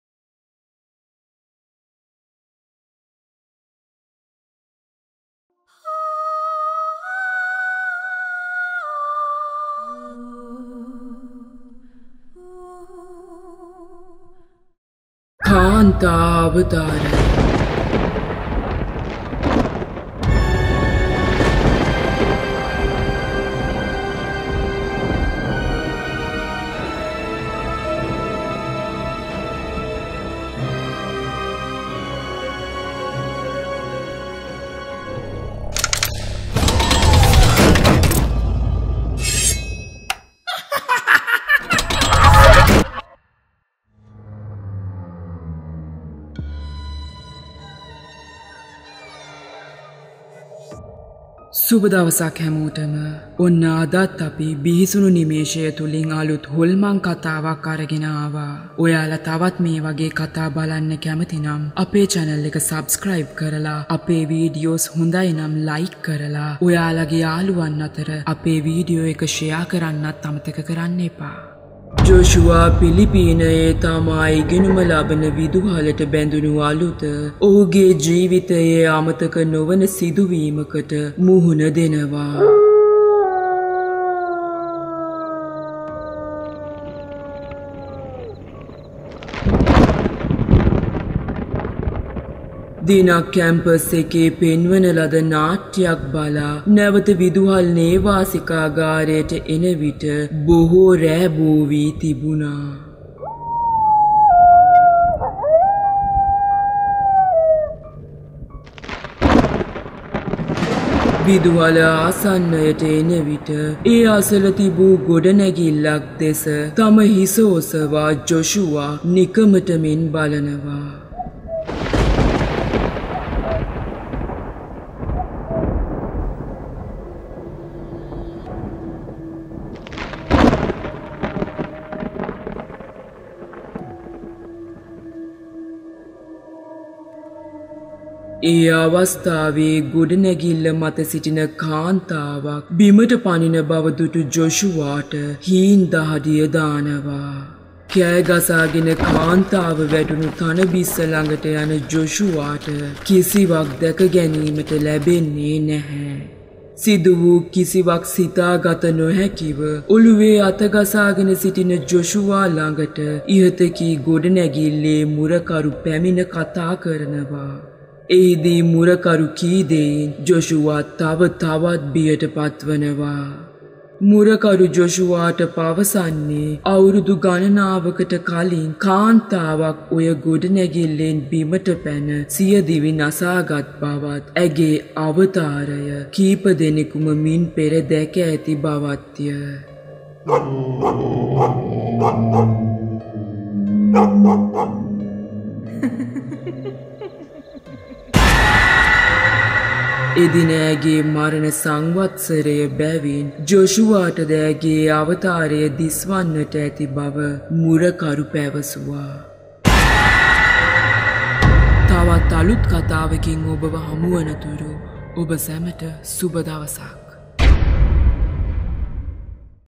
දිවි නසාගත් කාන්තා අවතාරය | creepy ghost story |True Horror Story | Holman katha | සත්‍ය හොල්මන් කතා
Joshua studies in a college in Phillipines. Oneday he got late to reach his hostel after watching a campus play. When he was walking alone he gets this weird urge to look up and he does so. He sees a lady jumping off a tall building. He runs towards the lawn to help that lady but to his surprise he finds nothing. What did he see? Was it real or?

The elements used in this video are computer animations.

Please be cautious while watching the video if you are a person who gets scared for the slightest thing.

වියාචනය

මෙම වීඩියෝ පටයේ භාවිතා කර ඇති අංග පරිගණක සජීවිකරණයන් වේ.

මෙම වීඩියෝව විනෝදය සඳහා පමණක් සාදා ඇත.

මෙම වීඩියෝව ළමයින් සඳහා නොවේ.

ඔබ සුළු දෙයටත් බිය වන පුද්ගලයෙක් නම් වීඩියෝව නරඹන විට සැලකිලිමත් වන්න.